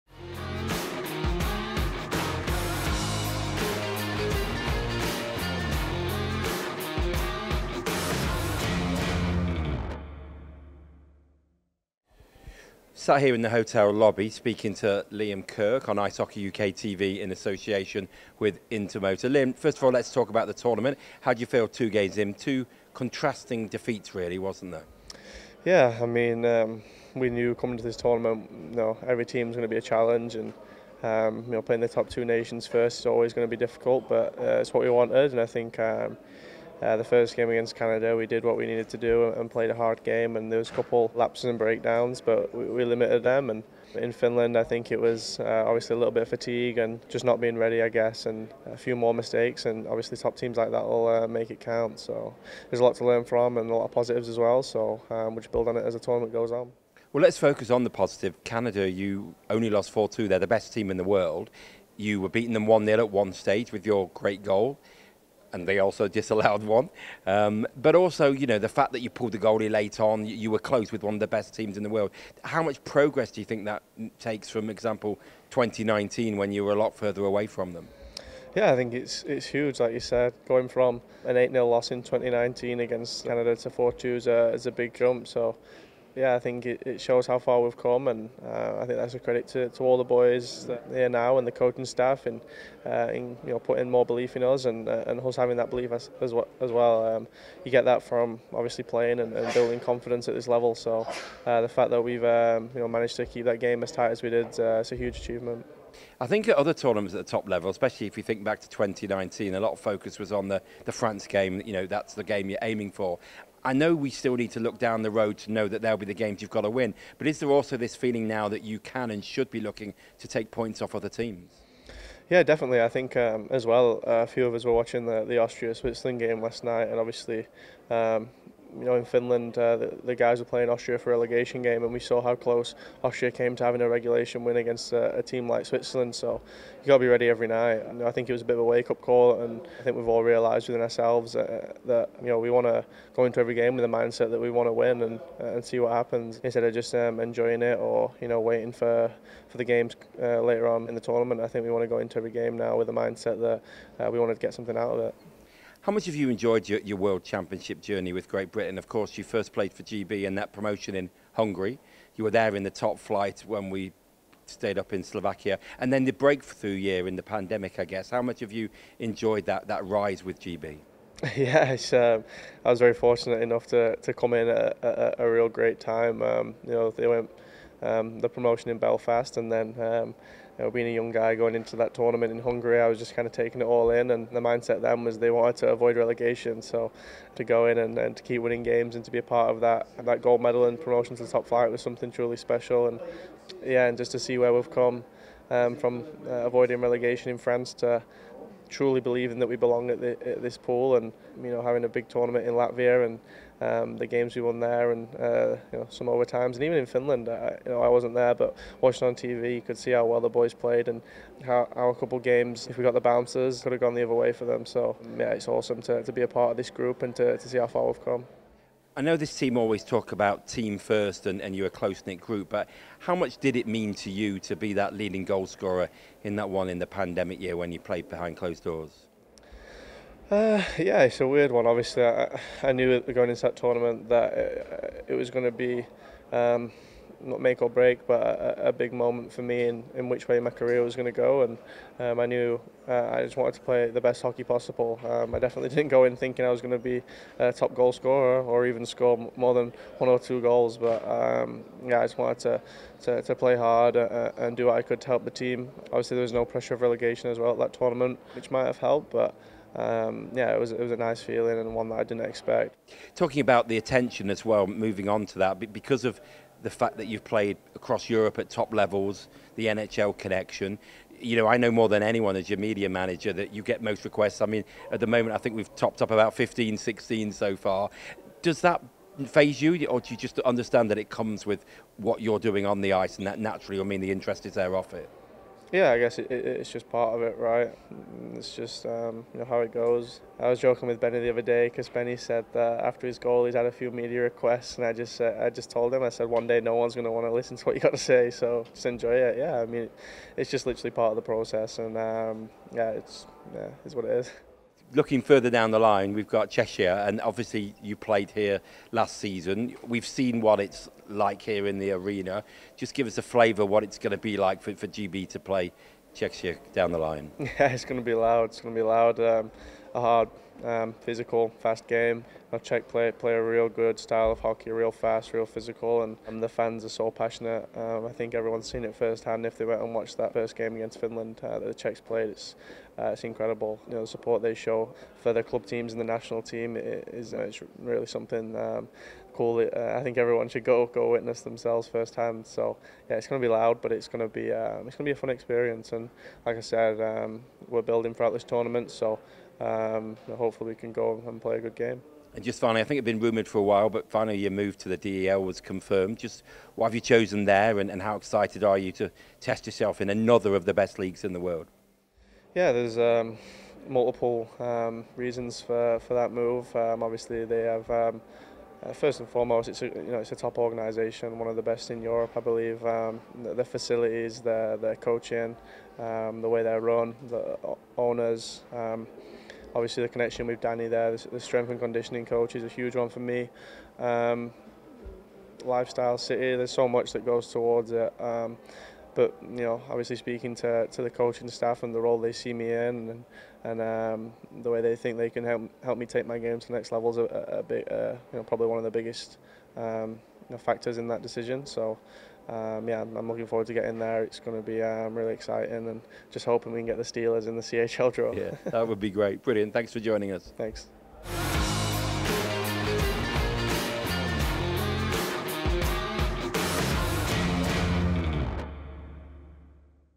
GB In Prague - 2024 World Championship - Liam Kirk interview
On Ice Hockey UK TV - in association with Intermotor - forward Liam Kirk reflects on Great Britain's start to the World Championship and look ahead to the rest of the tournament. He also reflects on his time so far as a GB player.